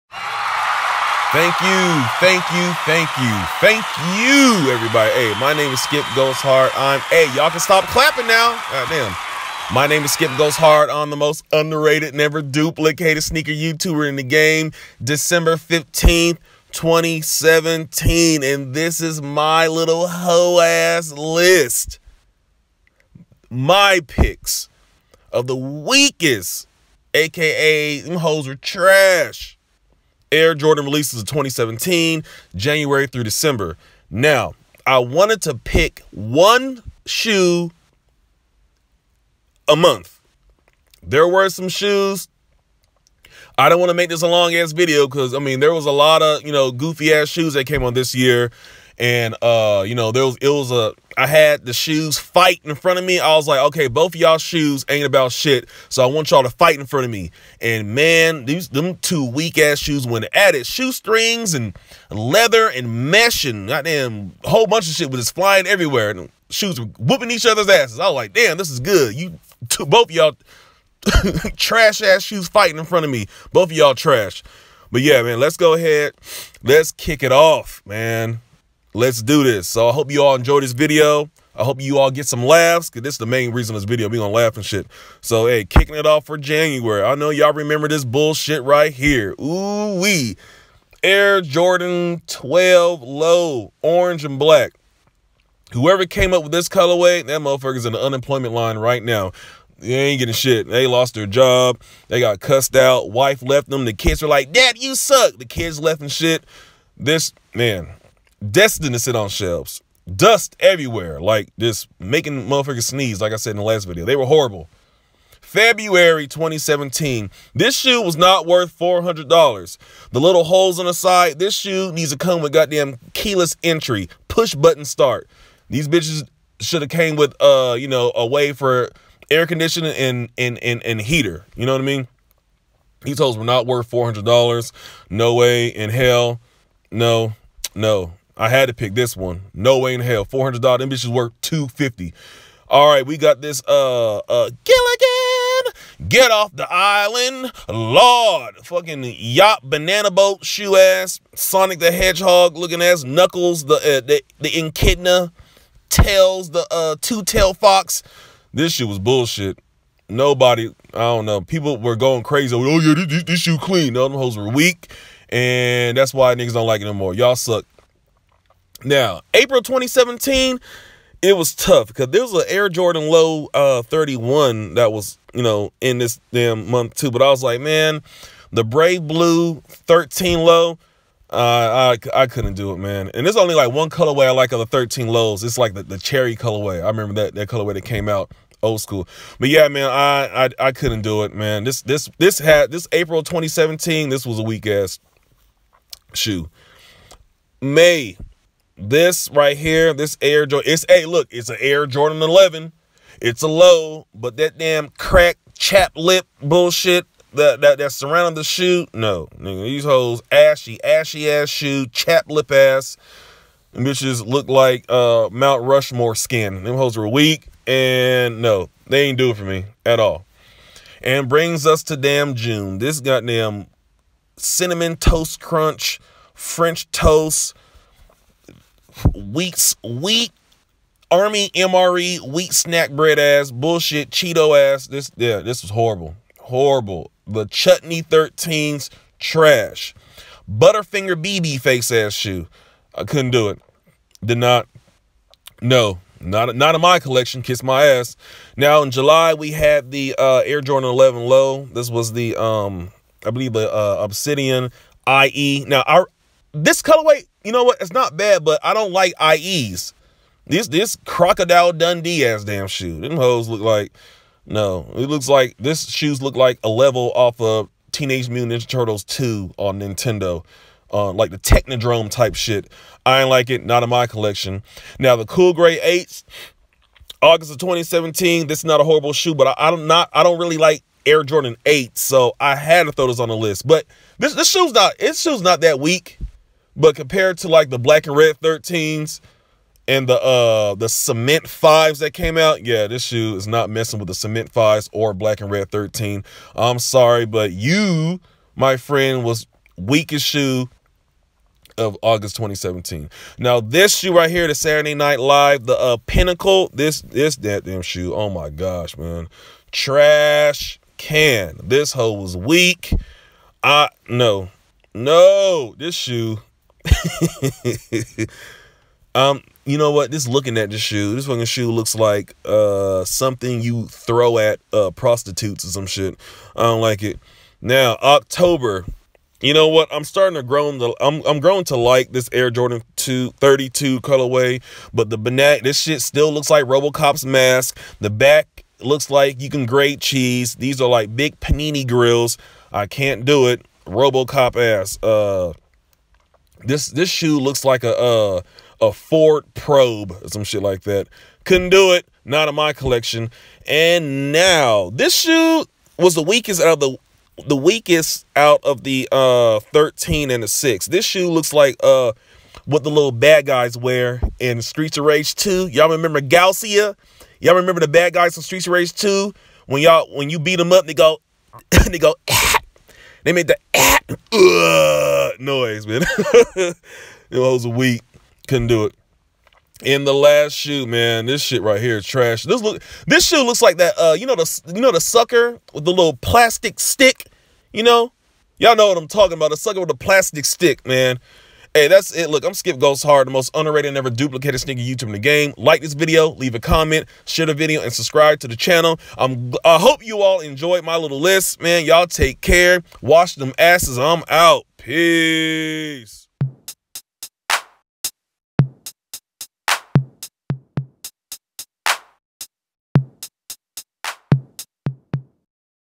TRASH! 2017 WEAKEST JORDAN RELEASES JANUARY-DECEMBER
Skip gives us his list of trash Jordan releases of 2017
THANKS!!